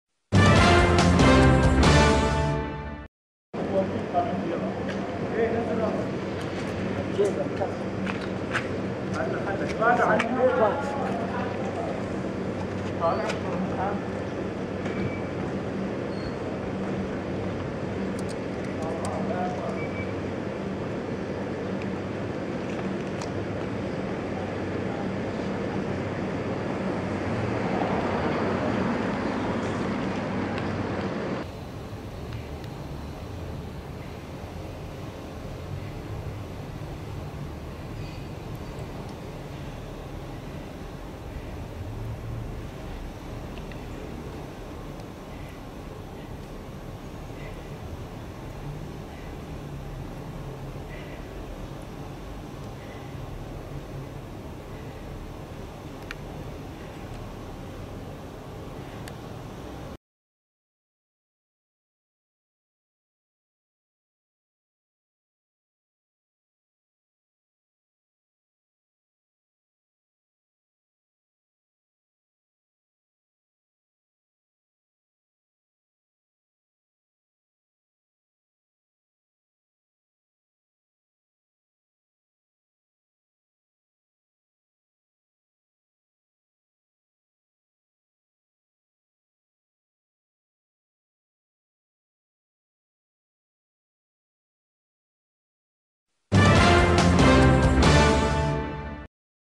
سند : إقتحام قوات النظام بعد إنتهاء مسيرة "آن الآن العصيان" 11/8/2013 | Bahrain
قامت قوات مرتزقة النظام الخليفي عصر يوم الأحد (11 أغسطس/آب 2013) بمحاولة عمل كمين غادر للمشاركين في مسيرة التعبئة التي دعا إليها إئتلاف شباب ثورة 14 فبراير تحت شعار "آن الآن العصيان"، عبر إقتحام المنطقة بِعشر مركبات من الخلف جهة المزارع.